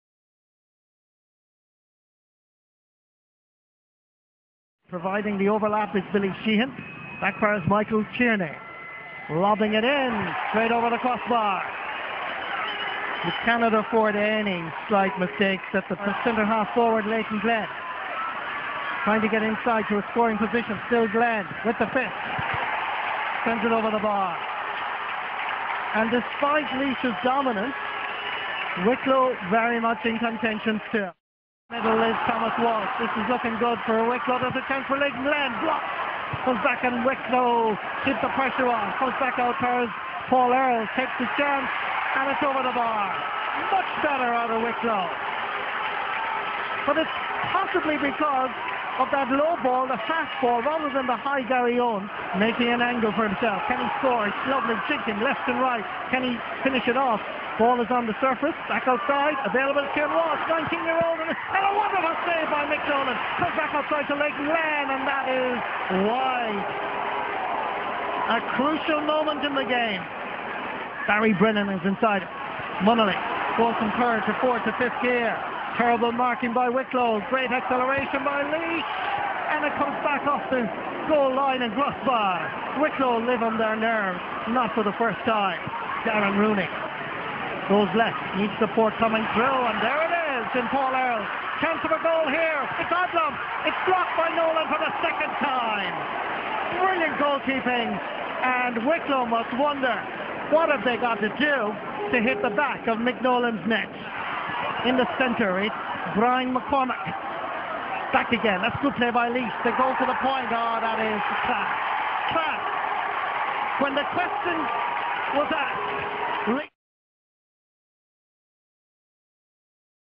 Wicklow V Laois Leinster championship football quarter final
Laois 0-15 Wicklow 0-13

31st May 2008

Croke Park